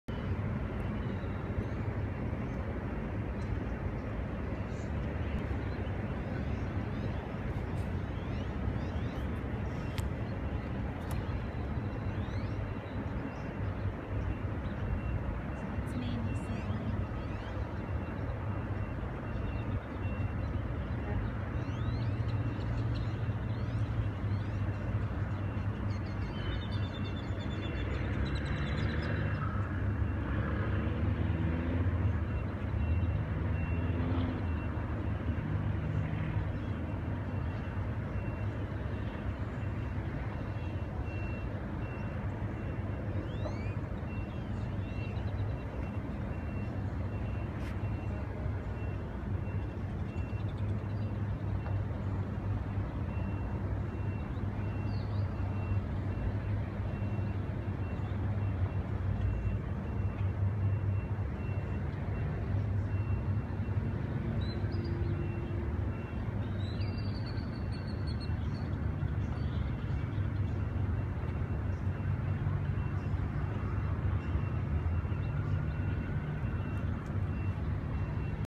Mandy Perfecting Her Sailing Skills
Senior Camp Counselor sailing with a Spring Break Camper on Lady Bird Lake